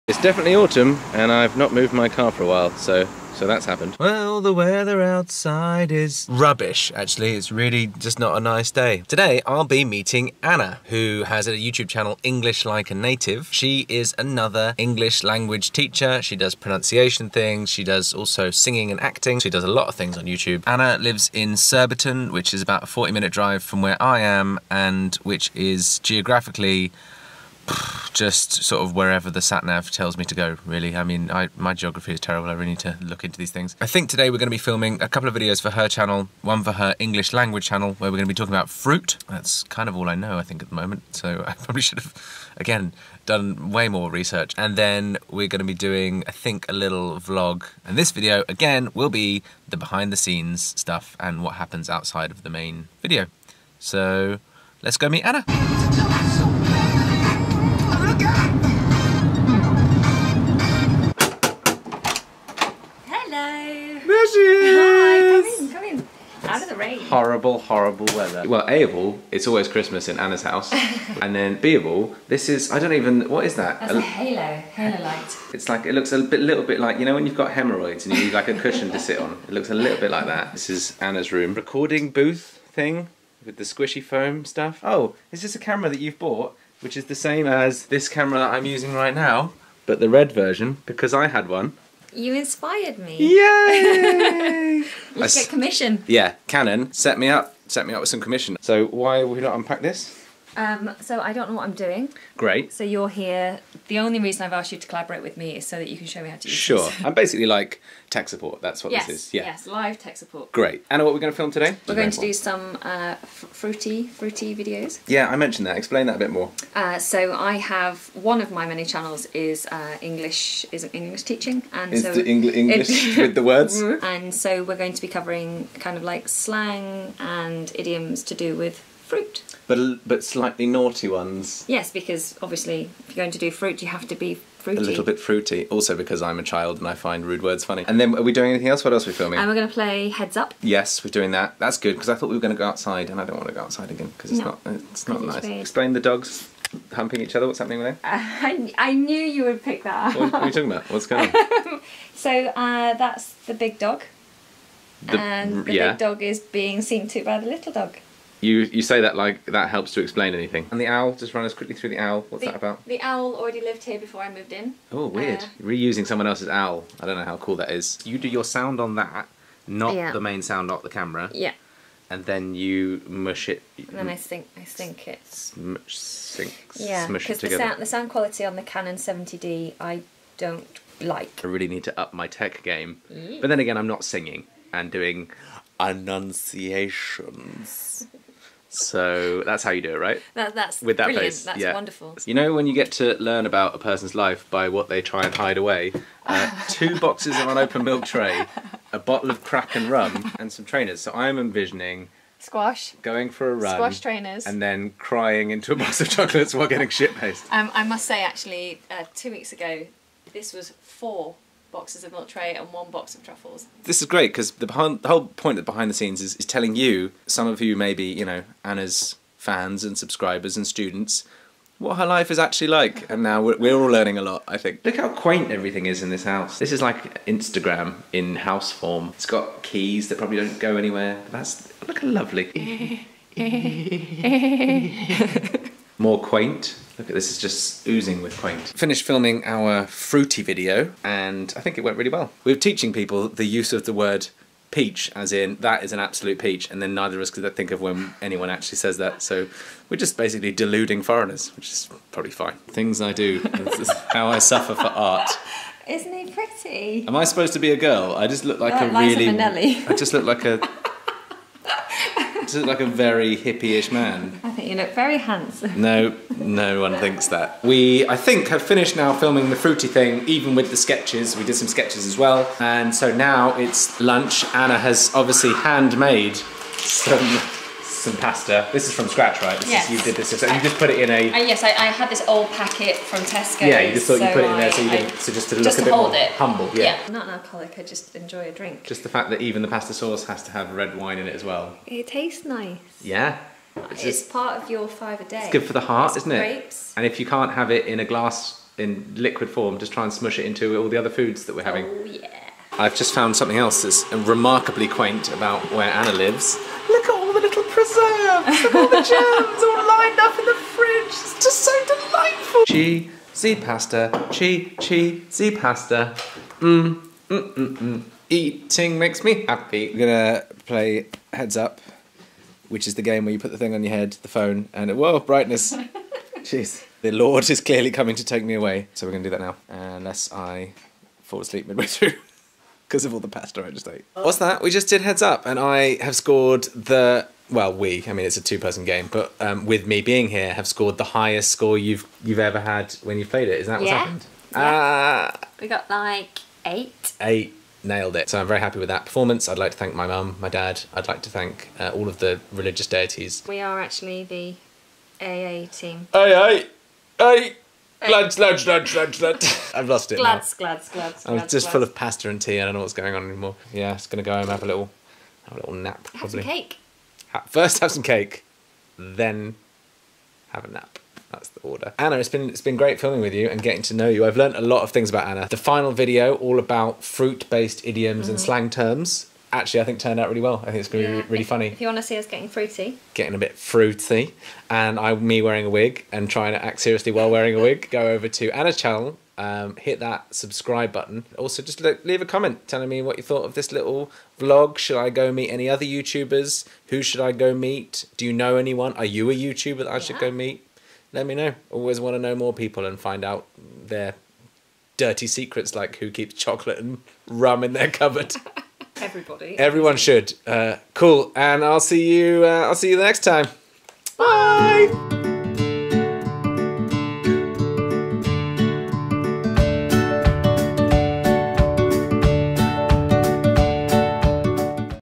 MEETING ANNA from English Like A Native | UK YouTuber
I toddled off to meet Anna, another UK YouTuber who makes English language learning videos, as well as vlogs, singing tutorials, and a host of other goodness.  Here's all the behind the scenes stuff, but make sure you go check out the finalised 'fruity' video on Anna's channel

FILMING SET UP
Camera: Canon Legria Mini - UK - US